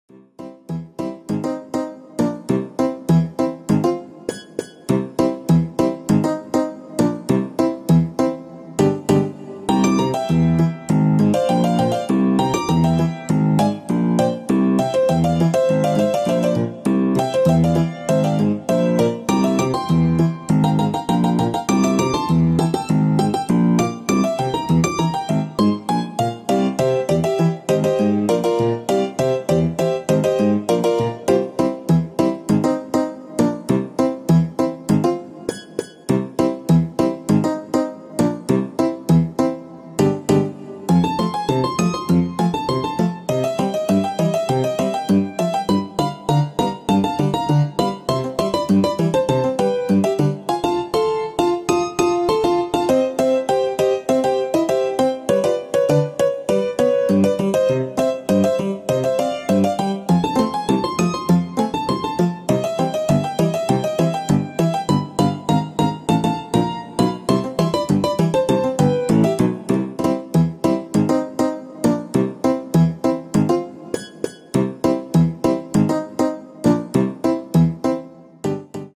Silent Comedy: The Mime Moving Mishap
In this hilarious silent comedy, watch as a spectator mocks a mime trapped inside an invisible box. The antics and misunderstandings lead to a series of laugh-out-loud moments that you won't want to miss. Join the fun and see how the mime's clever tricks and the spectator's playful teasing create a comedic adventure!